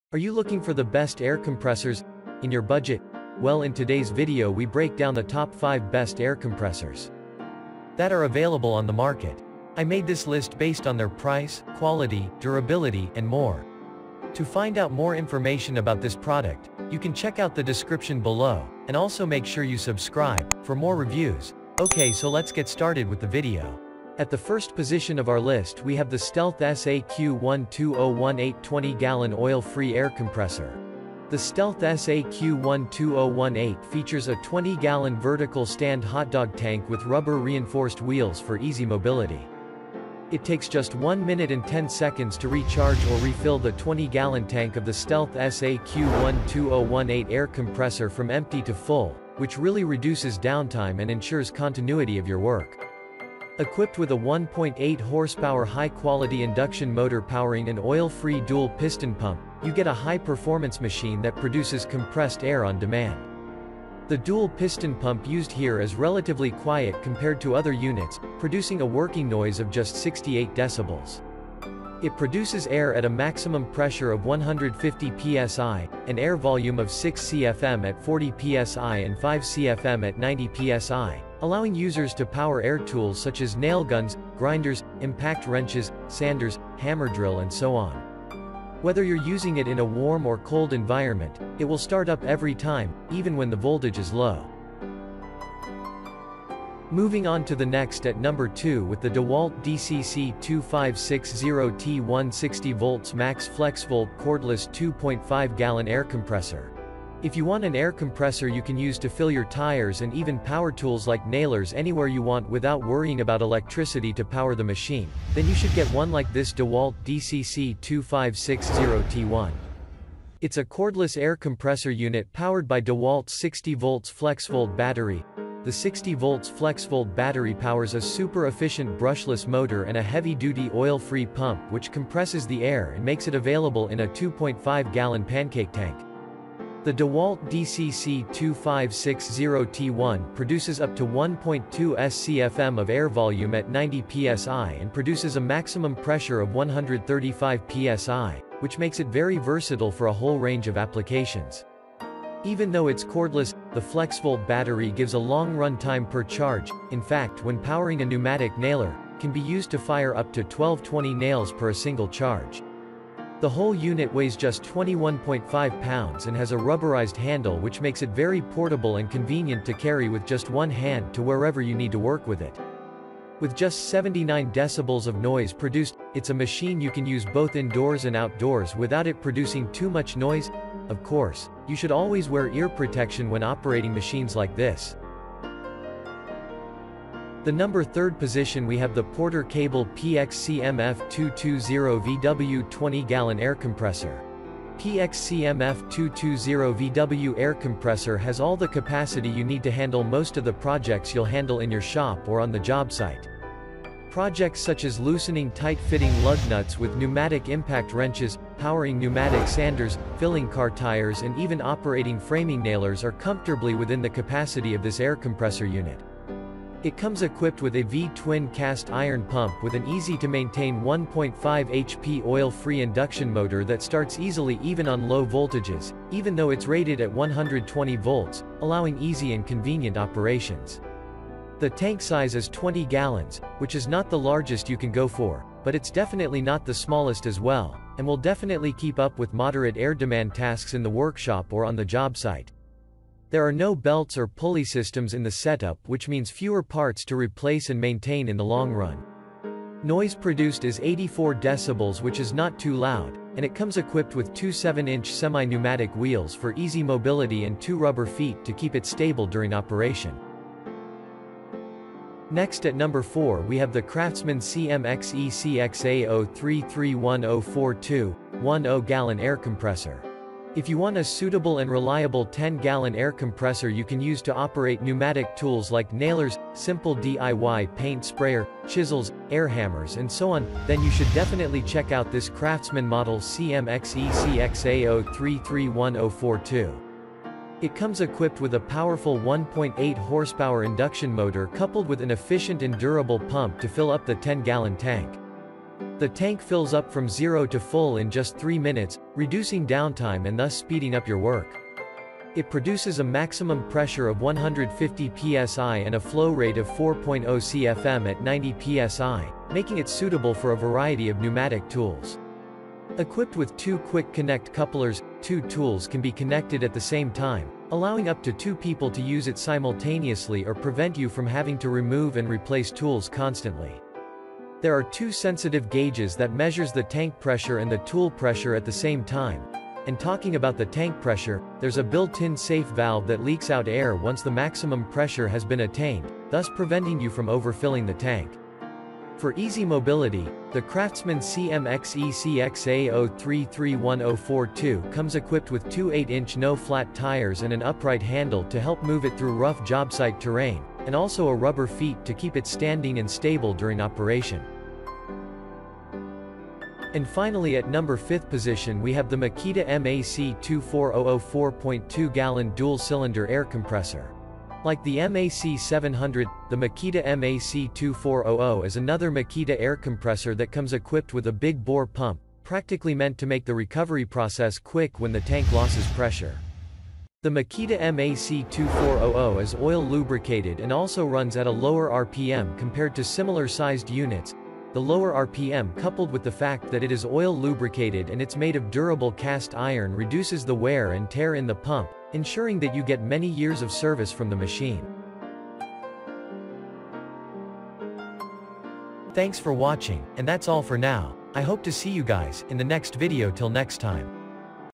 ✅Top 5 Best Air Compressors For Powering Pneumatic Tools of 2023
✅1. Stealth SAQ-12018 20 Gallon Oil-Free Air Compressor
✅2. Dewalt DCC2560T1 60V Max Flexvolt Cordless 2.5 Gallon Air Compressor
✅3. Porter Cable PXCMF220VW 20 Gallon Air Compressor
✅4. Craftsman CMXECXA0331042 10 Gallon Air Compressor
✅5. Makita MAC2400 4.2 Gallon Dual-Cylinder Air Compressor

To save you both time and money, we’ve narrowed down to some of the best Air Compressors.
Check out an in-depth review of the best Air Compressors in 2022 .

In this video, we make a new research on the top best Air Compressors.
This is the best Air Compressors review.
Best time for buying your new Air Compressors .